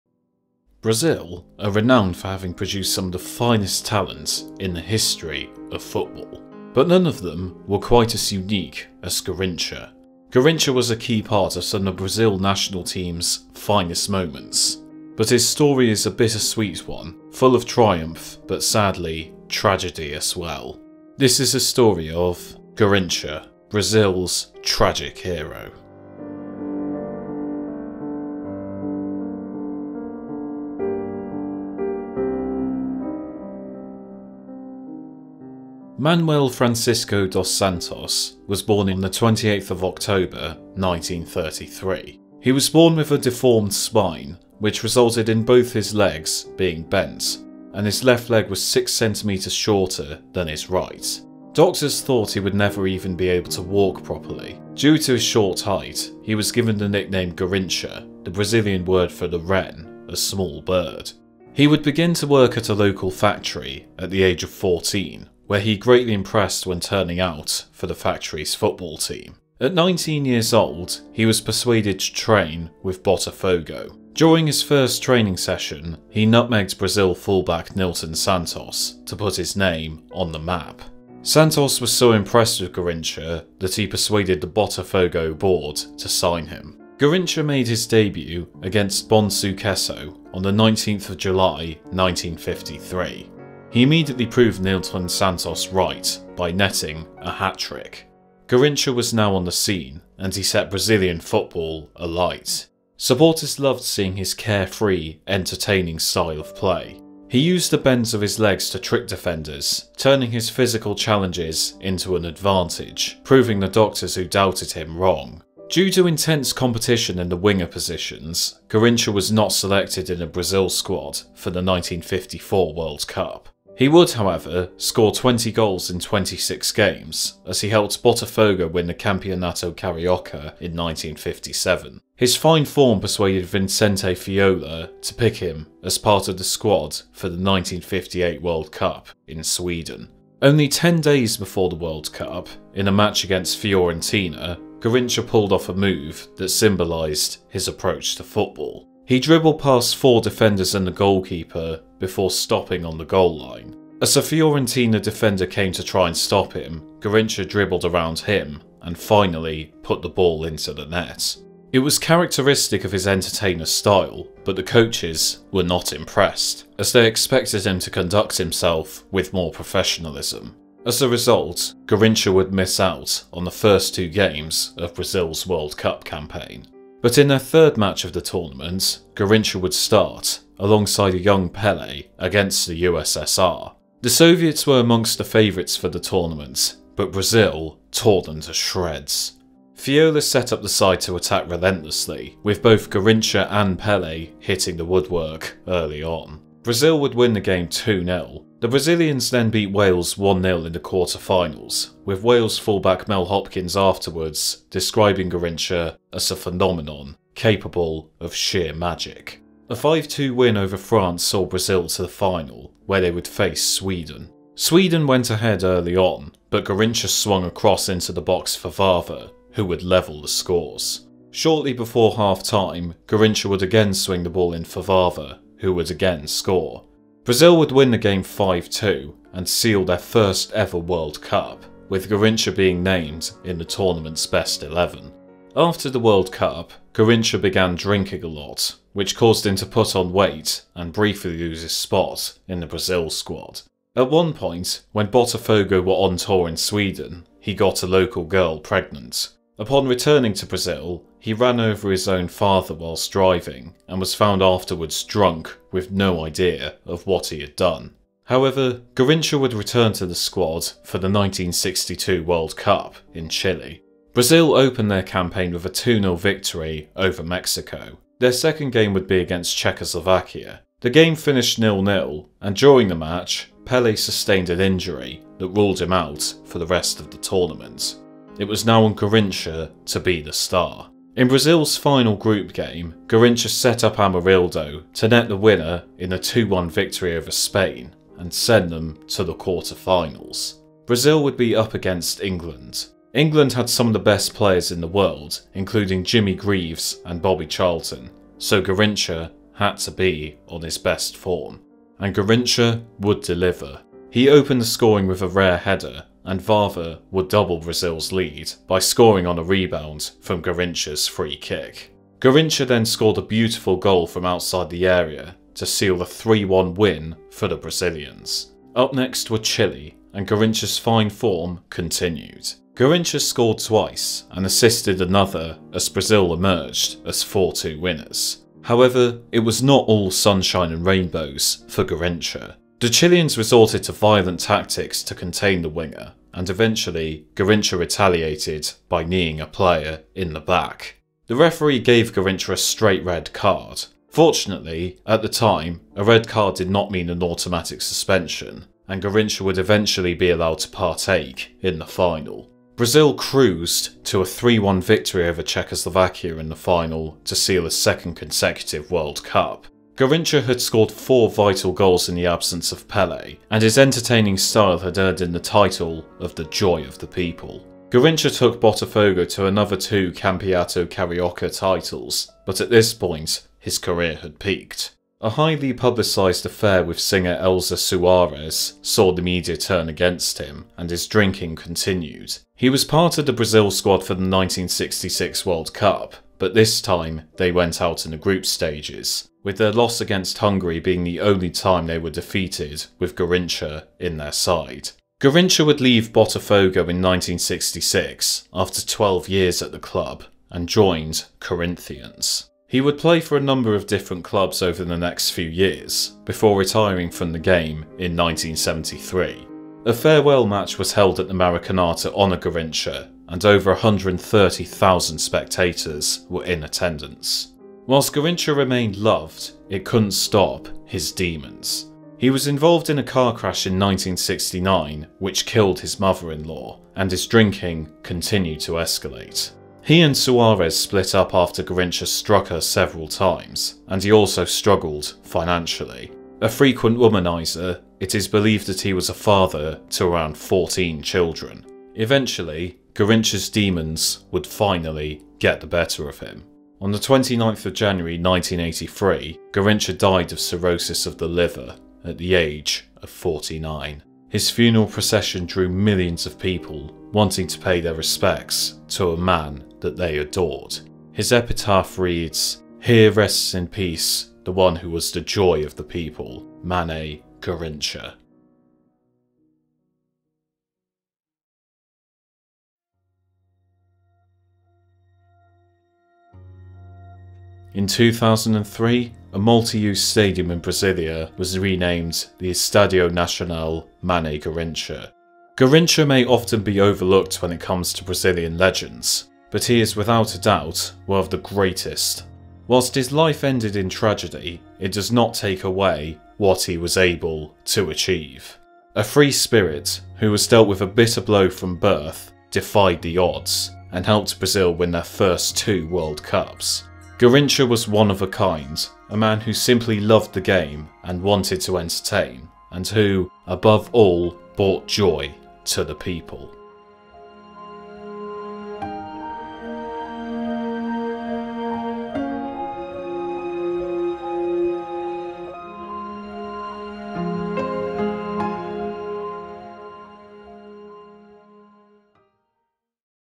Garrincha-Brazil's Tragic Hero | AFC Finners | Football History Documentary
Garrincha is one of the finest players to ever emerge from Brazil. He dazzled spectators with his unique style of play, defying the odds given to him at an early age. But sadly, for all his success, his life off the pitch was riddled with demons that eventually got the better of him.

Sources-